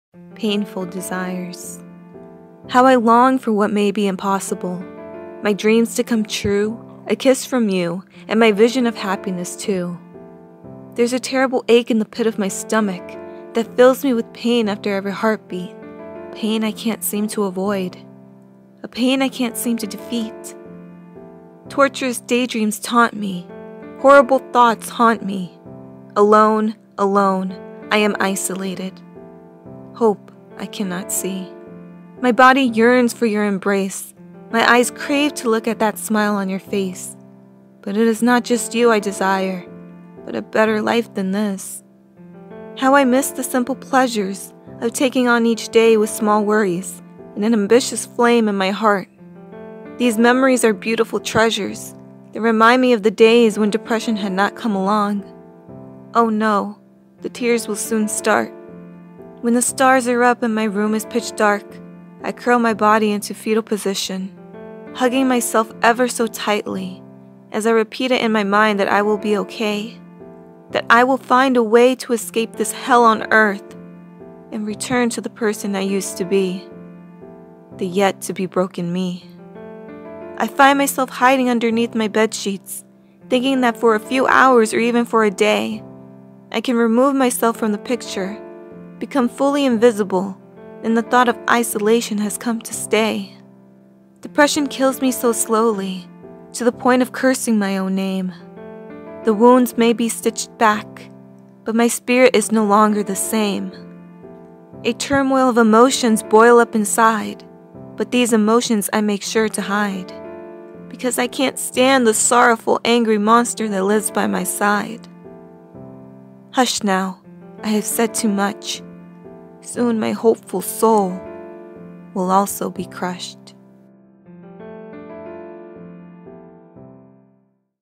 Painful Desires │Spoken Word Poetry
When we wish and hope for certain things and either have yet to obtain them or believe we never will have them, what were once euphoric desires become painful desires.

Written & Performed by: Melissa Paiz

Background music from freesound.org by the user meral